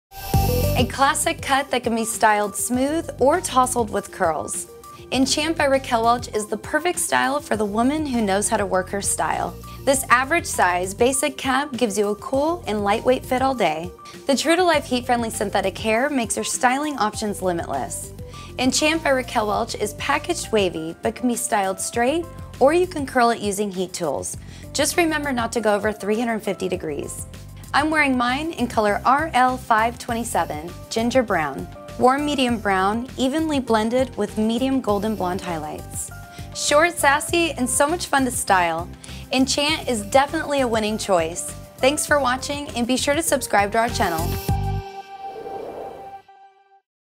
Enchant Wig by RAQUEL WELCH
A classic cut that can be styled smooth or with tousled curls. Tru2Lifeheat friendly synthetic hair makes your styling options limitless! The Enchant Wig by Raquel Welch is packaged wavy and can be straightened or curled using hair tools.